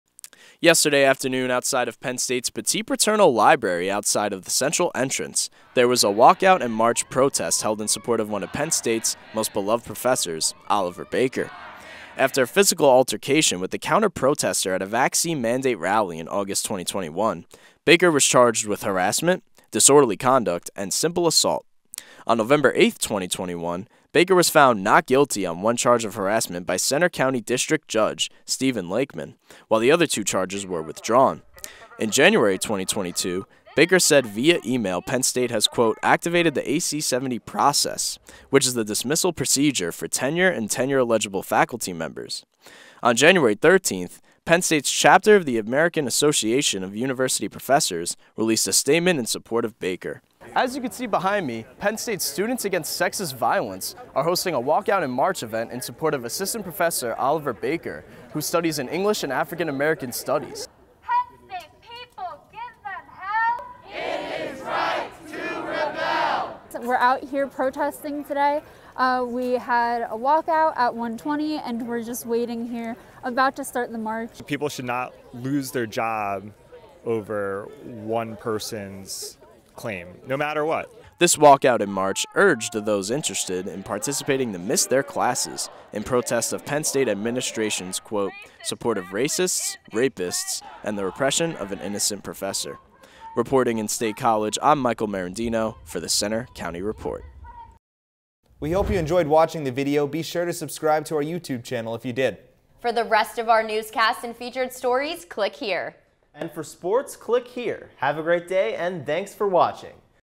Penn State Student Protest Supports Professor
Some Penn State students held a walkout and protest to support a professor charged, then cleared of criminal charges, after a physical altercation. Micael Merendino reports.